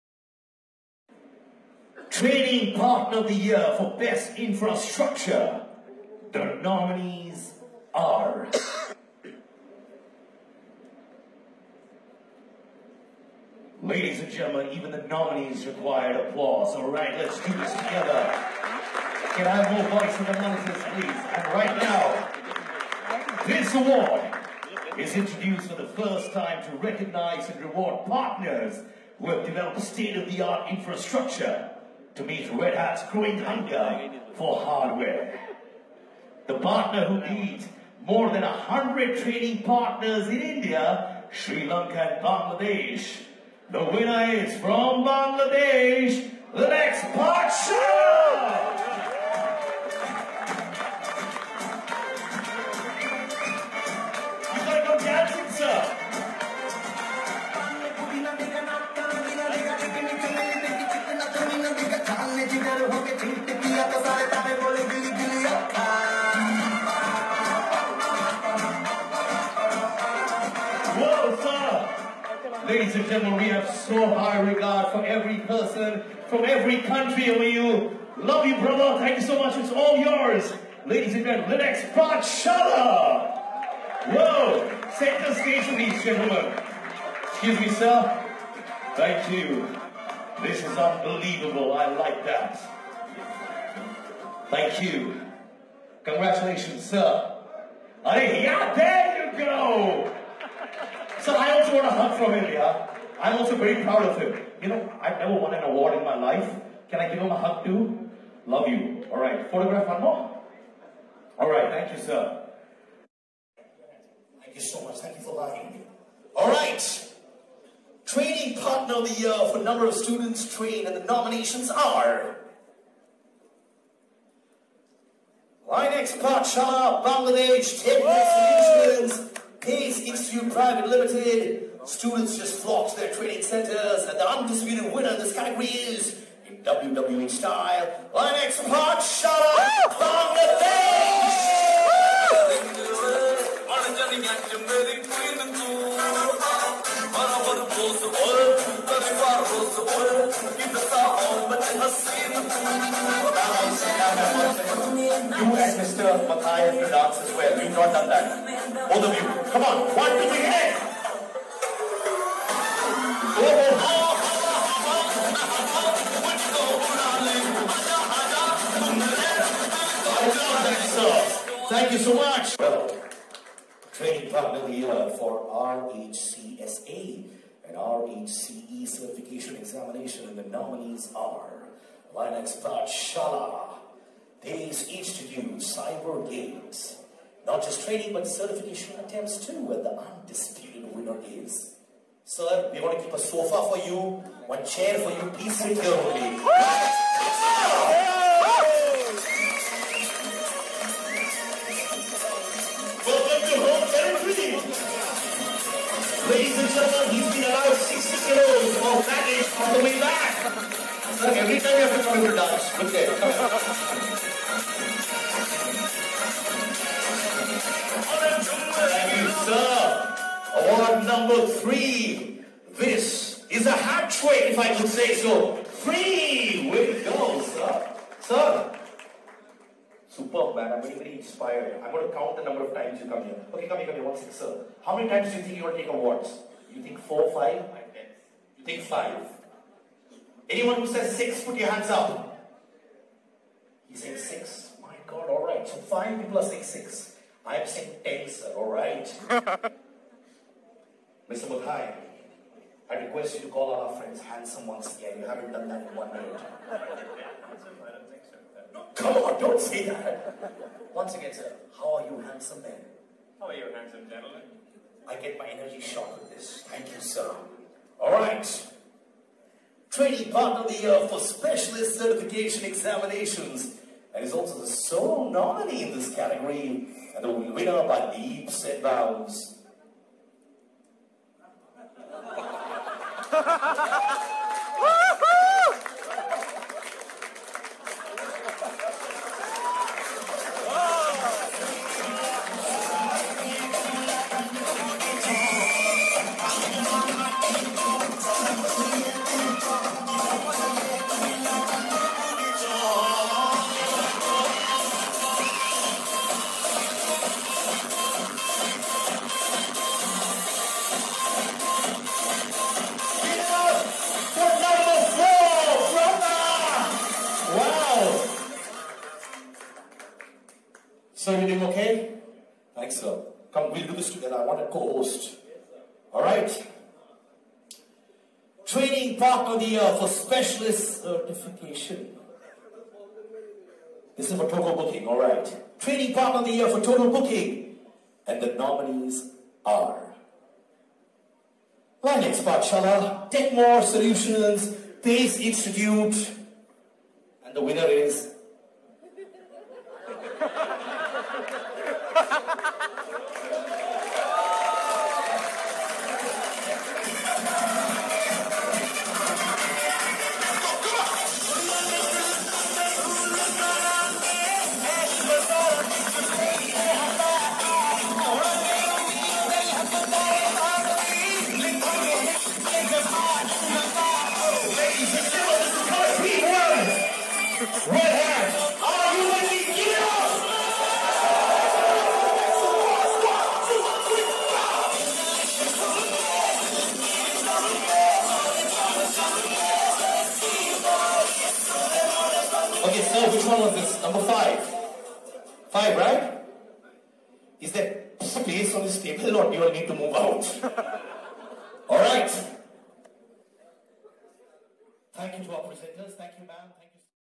Linux Pathshala Received 5 Red Hat Awards in 2018
1.Best Infrastructure ( Based on Hardware) among more than 100 RedHat partners in Indian region.

2.RedHat Partner of the year for Maximum student Trained among Bangladesh, Srilanka and Bhutan.

3. RedHat Partner of the year for Maximum RHCSA & RHCE Certification Examinations among Bangladesh, Srilanka and Bhutan

4. RedHat Partner of the year for Maximum Specialist Certification Examinations among Bangladesh, Srilanka and Bhutan.

5. Partner of the year for TOTAL BOOKING.